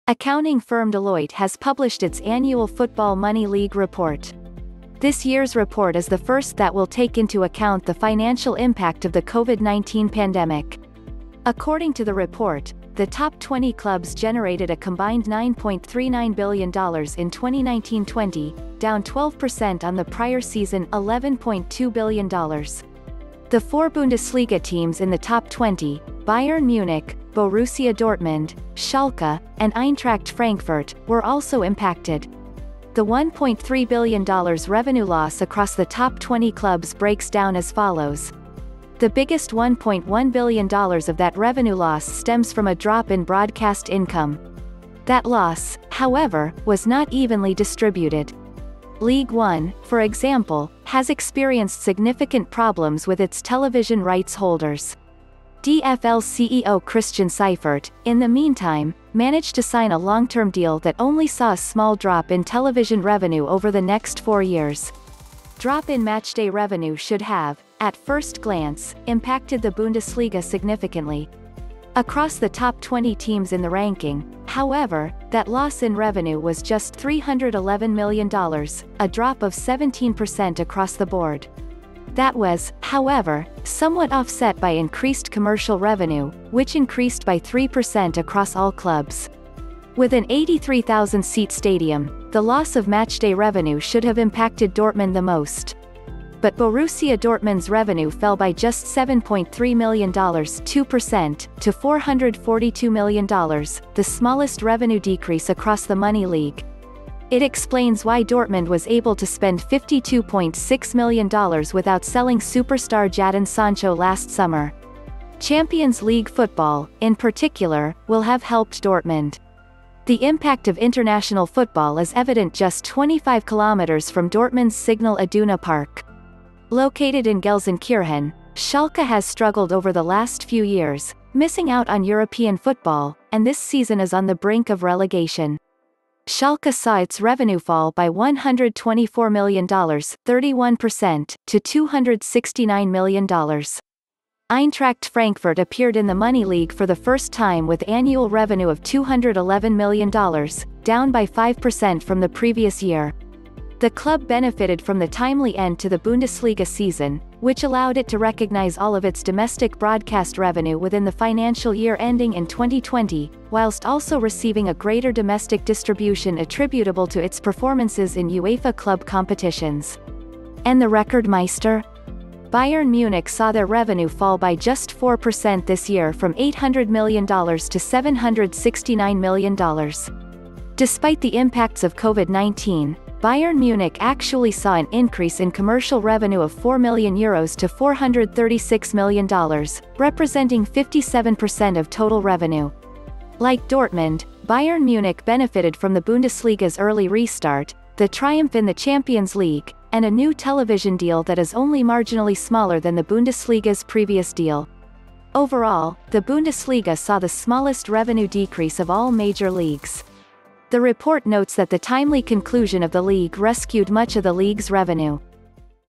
Deloitte Report Bundesliga COVID 19 Restart Limited Financial Impact
Accounting firm Deloitte has published its annual Football Money League report. This year's report is the first that will take into account the financial impact of the Covid-19 pandemic. According to the report, the top 20 clubs generated a combined $9.39 billion in 2019/20, down 12% on the prior season ($11.2 billion). The four Bundesliga teams in the top 20, Bayern Munich, Borussia Dortmund, Schalke, and Eintracht Frankfurt, were also impacted. The $1.3 billion revenue loss across the top 20 clubs breaks down as follows. The biggest $1.1 billion of that revenue loss stems from a drop in broadcast income. That loss, however, was not evenly distributed. Ligue 1, for example, has experienced significant problems with its television rights holders. DFL CEO Christian Seifert, in the meantime, managed to sign a long-term deal that only saw a small drop in television revenue over the next four years. Drop-in matchday revenue should have, at first glance, impacted the Bundesliga significantly. Across the top 20 teams in the ranking, however, that loss in revenue was just $311m, a drop of 17% across the board. That was, however, somewhat offset by increased commercial revenue, which increased by 3% across all clubs. With an 83,000 seat stadium, the loss of matchday revenue should have impacted Dortmund the most. But Borussia Dortmund’s revenue fell by just $7.3m (2%) to $442m, the smallest revenue decrease across the Money League. It explains why Dortmund was able to spend $52.6 million without selling superstar Jadon Sancho last summer. Champions League football, in particular, will have helped Dortmund. The impact of international football is evident just 25km from Dortmund's SIGNAL IDUNA Park. Located in Gelsenkirchen, Schalke has struggled over the last few years, missing out on European football, and this season is on the brink of relegation. Schalke saw its revenue fall by $124m (31%) to $269m. Eintracht Frankfurt appeared in the Money League for the first time with annual revenue of $211m, down by 5% from the previous year. The club benefitted from the timely end to the Bundesliga season, which allowed it to recognize all of its domestic broadcast revenue within the financial year ending in 2020, whilst also receiving a greater domestic distribution attributable to its performances in UEFA club competitions. And the Rekordmeister? Bayern Munich saw their revenue fall by just 4% this year from $800m to $769m. Despite the impacts of COVID-19, Bayern Munich actually saw an increase in commercial revenue of €4m to $436m, representing 57% of total revenue. Like Dortmund, Bayern Munich benefitted from the Bundesliga's early restart, the triumph in the Champions League, and a new television deal that is only marginally smaller than the Bundesliga's previous deal. Overall, the Bundesliga saw the smallest revenue decrease of all major leagues. The report notes that the timely conclusion of the league rescued much of the league's revenue.